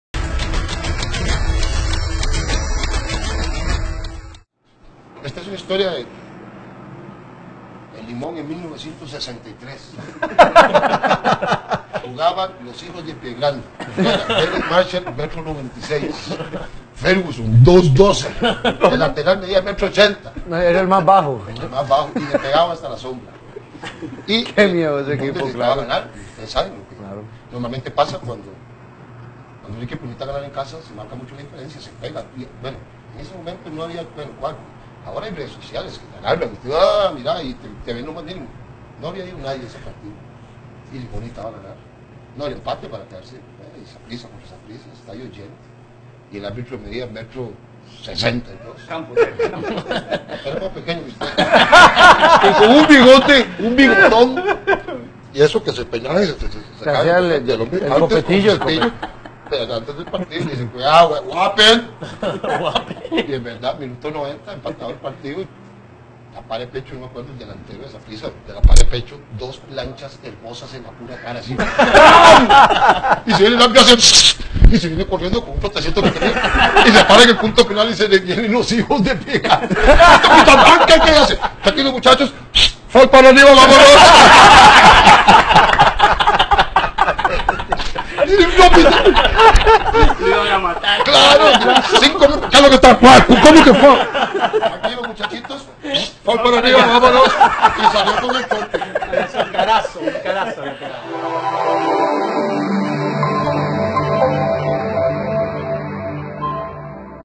David Diach - Anécdota Arbitro con los hijos de pie Grande.
Créditos programa VIS10N -Alonso-Andrey-Atim
Invitados David Diach y Diego País
Anecdota Partido en Limón Arbitro y los hijos de Pie Grande.😆😆😆